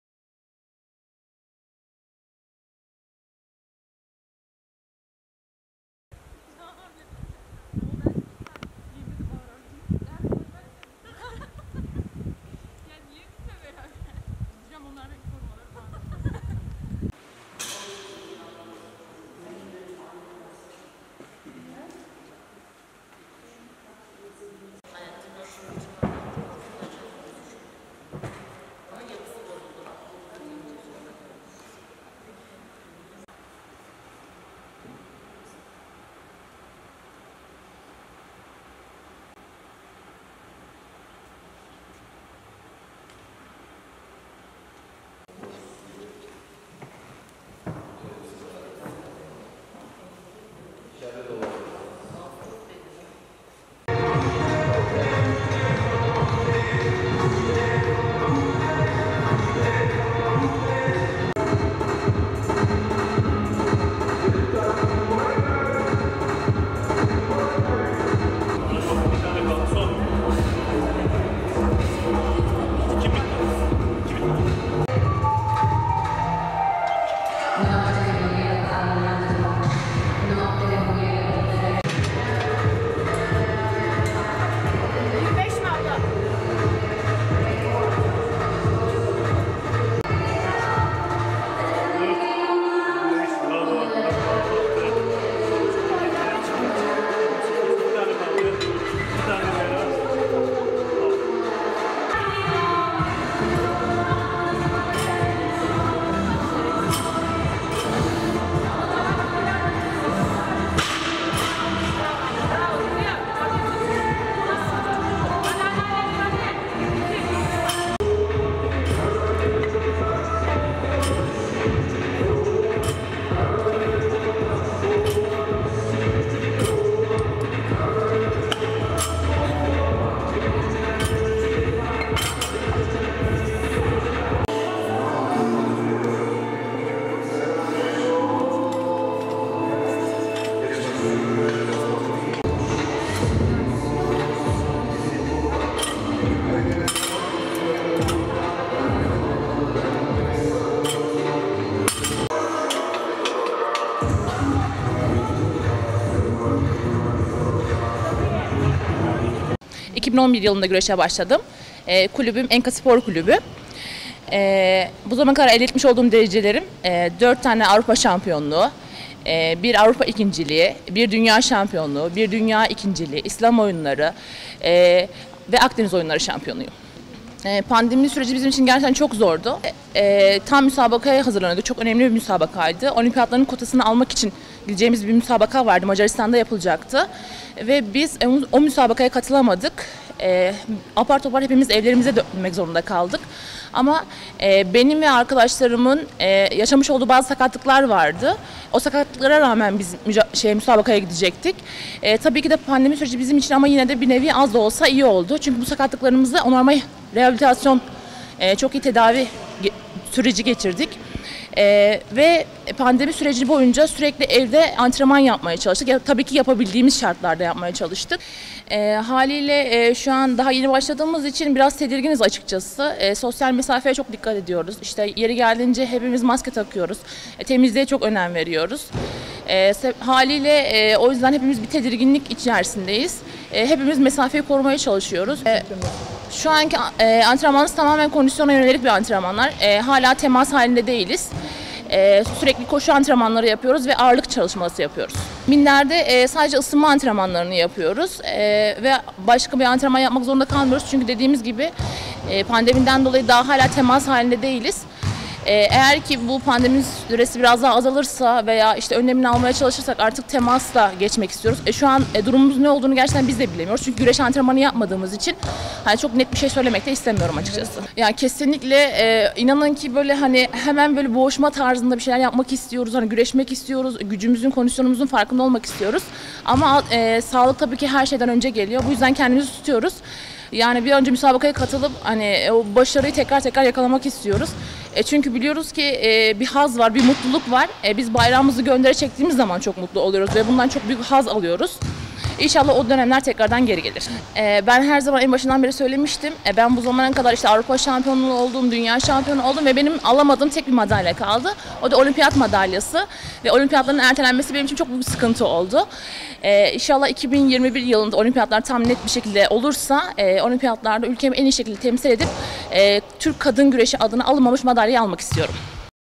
Milli güreşçi Yasemin Adar, olimpiyatlara adını altın harflerle yazdırmak istiyor - BOLU
GÖRÜNTÜ DÖKÜMÜ :
- Adar'ın antrenmanından detay görüntüler
- Dünya şampiyonu milli güreşçi Yasemin Adar ile röportaj
 Milli güreşçi Yasemin Adar, olimpiyatlara adını altın harflerle yazdırmak istiyor
- Dünya şampiyonu milli güreşçi Yasemin Adar:
- "İnşallah 2021 yılında olimpiyatlar olursa orada ülkemi en iyi şekilde temsil edip, Türk kadını adına güreş alanında alınmamış madalyayı almak istiyorum"
BOLU (AA) - ZAFER GÖDER - Dünya şampiyonu milli güreşçi Yasemin Adar, olimpiyatlara adını altın harflerle yazdırmak için çalışmalarını sürdürüyor.

Bolu'nun Aladağ bölgesindeki Kamp Eğitim Merkezi'nde çalışmalarını sürdüren Kadın Güreş Milli Takımı'nda yer alan Yasemin, günde çift antrenman yaparak 2021'e ertelenen 2020 Tokyo Olimpiyatları'na hazırlanıyor.

Yeni tip koronavirüs (Kovid-19) tedbirleri kapsamında minderden uzak kalan milli güreşçi, kondisyonunu yükseltmek için kampta güç depoluyor.

- "Mindere çıkmayı bekliyoruz"

Yasemin Adar, AA muhabirine yaptığı açıklamada, 2011 yılında başladığını spor hayatına 1 dünya şampiyonluğu, 1 dünya ikinciliği, 4 Avrupa şampiyonluğu, 1 Avrupa ikinciliği, İslam Oyunları ve Akdeniz Oyunları şampiyonluğu sığdırdığını hatırlatarak, bundan sonraki en büyük hedefinin olimpiyat madalyası kazanmak olduğunu söyledi.

Yasemin, pandemi sürecinin kendileri için zor geçtiğini dile getirerek, "Tam müsabakalara hazırlanıyorduk. Olimpiyat kotası almak için gideceğimiz bir müsabaka vardı Macaristan'da. O müsabakaya katılamadık. Apar topar hepimiz evlerimize dönmek zorunda kaldık. Öte yandan benim ve arkadaşlarımın bazı sakatlıkları vardı. O sakatlıklara rağmen müsabakaya gidecektik. Bu nedenle pandemi süreci bizim için bir nebze iyi oldu çünkü sakatlıklarımızın iyileşmesi açısından çok iyi tedavi süreci geçirdik. Pandemi süreci boyunca evde yapabildiğimiz şartlarda antrenman yapmaya çalıştık." diye konuştu.

 Kontrolü sosyal hayatın başlamasıyla milli takımla kampa katıldığını anlatan 28 yaşındaki güreşçi, "Kampımıza yeni başladığımız için biraz tedirginiz. Sosyal mesafeye çok dikkat ediyoruz. Yeri gelince maske takıyoruz. Temizliğe çok önem veriyoruz." dedi.

Yasemin Adar, kampta pandemi nedeniyle henüz mindere çıkamadıklarını aktararak, şöyle devam etti:

"Antrenmanımız kondisyona yönelik devam ediyor. Hala temas halinde değiliz. Sürekli koşu antrenmanları ve ağırlık çalışması yapıyoruz. Minderde sadece ısınma antrenmanları yapıyoruz. Güreş antrenmanı yapmadığımız için durumumuz hakkında çok net bir şey söylemek de istemiyorum. Mindere çıkmayı bekliyoruz. Güreşmek istiyoruz. Gücümüzün, kondisyonumuzun farkında olmak istiyoruz. Ancak sağlık her şeyden önce geliyor. Bu yüzden kendimizi tutuyoruz."

Bir an önce müsabakalara çıkıp yeni başarılar elde etmek istediklerini belirten Yasemin, "Bayrağımızı göndere çektirdiğimiz zaman çok mutlu oluyoruz. İnşallah o dönemler tekrar geri gelir. Alamadığım tek bir madalya kaldı; o da olimpiyat madalyası. Olimpiyatların ertelenmesi benim için çok büyük sıkıntı oldu. İnşallah 2021 yılında olimpiyatlar olursa orada ülkemi en iyi şekilde temsil edip Türk kadını adına güreş alanında alınmamış madalyayı almak istiyorum." ifadelerini kullandı.